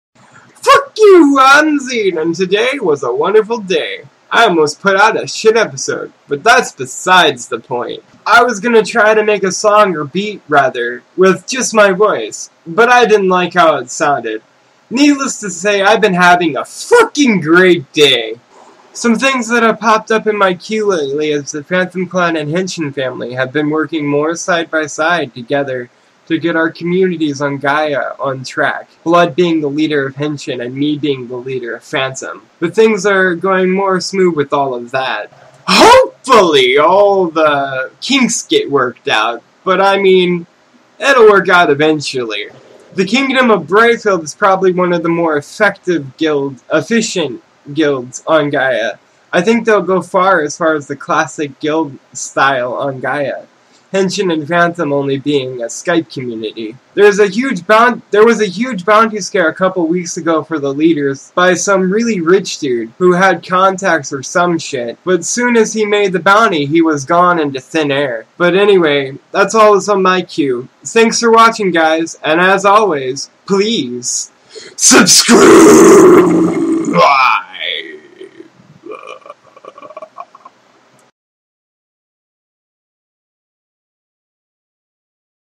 Fuck You I'm Zeeden show ep22
Fuck you i'm zeeden and today was a wonderful day i almost put out a shit episode
but that's besides the point, i was going to try to make a song or beat rather, with just my voice. but i didn't like how it sounded.
needless to say i have been having a fucking grate day!
some things that have popped up on my que lately is that phantom clan and henshin family
have been working more side by side to try to get our community's on gaia on track
blood being the leader of henshin and me being the leader of phantom.
but things are going more smooth with all of that.  hopefully all the kinks work out soon.
the kingdom of bright field being probably one of the more efficient guilds on gaia
i think they will go far with the hole classic guild style. henshin and phantom being skype communities
there was a huge bounty scare a couple weeks ago for all the leaders heads by some really rich dude who had connects or some shit. but as soon as he made the bounty he was gone into thin air.